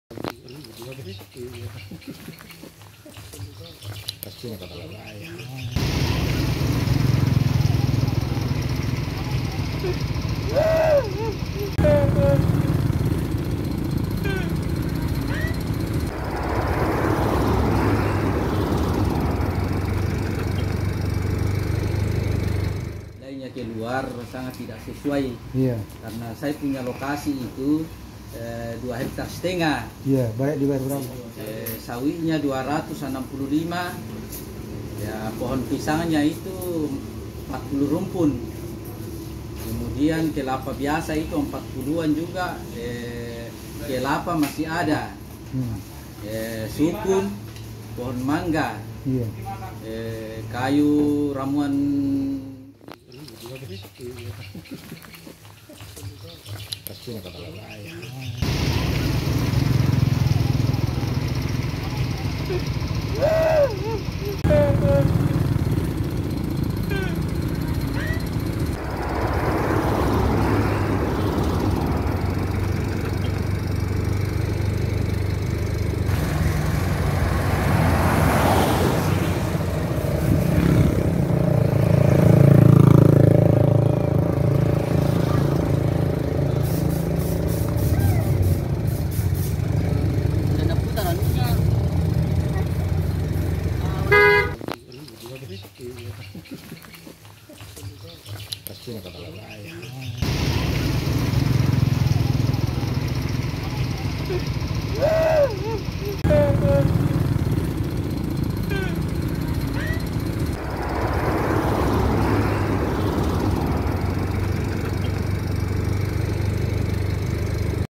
Merasa Dirugikan Warga Tolak Ganti Rugi Lahan Bendungan di Mateng 2,5 Hektare Hanya Dibayar Rp 57 Ju
TRIBUN-SULBAR.COM,MAMUJU-Ramli (48) warga Desa Batu Parigi, Kecamatan Tobadak, Kabupaten Mamuju Tengah,Sulawesi Barat (Sulbar), menolak ganti rugi lahan pembangunan proyek Bendungan Budong-budong, Mamuju Tengah (Mateng)

Pasalnya lahan produktif miliknya hanya dihargai senilai Rp 57 juta dengan luas 2,5 hektare.

Menurut Ramli seharunsya nilai pembayaran lahan miliknya itu sebesar Rp 250 juta bukan malah dibeli harga murah.

"Saya tidak menerima (menolak) pembayaran lahan itu, karena saya merasa sangat dirugikan kalau mau dibayar dengan Rp 57 juta," ungkap Ramli saat mengadu di Kantor Satuan Kerja Balai Sungai Wilayah 3 Palu, Jl Martadinata, Mamuju, Kamis (19/1/2023).

Laporan Waratawan Tribun-Sulbar.com Abd Rahman